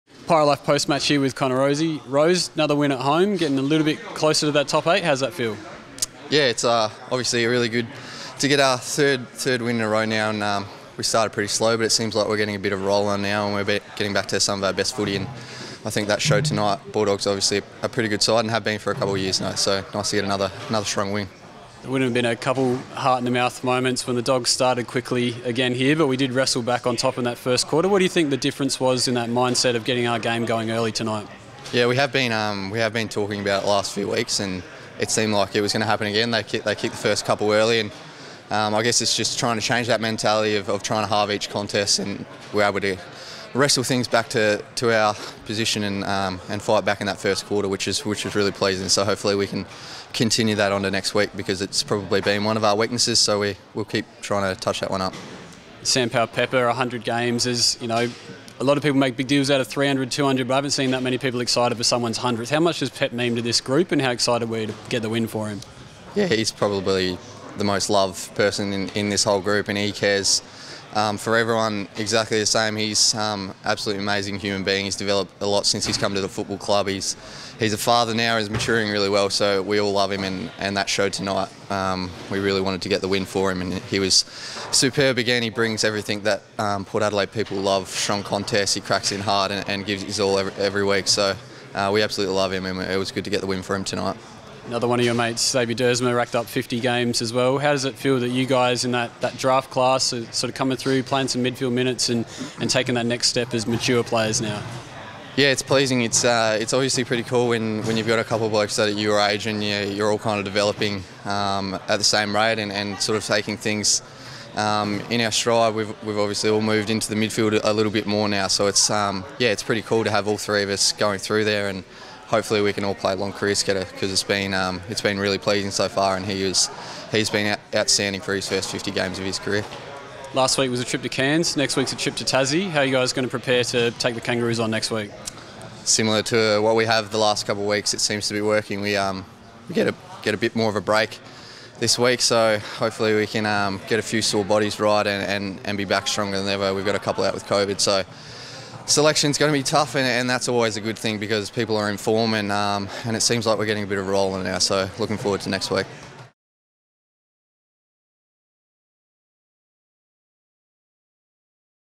Pirate Life Post Match: Connor Rozee
We speak with Connor Rozee after our Round 8 win against the Bulldogs.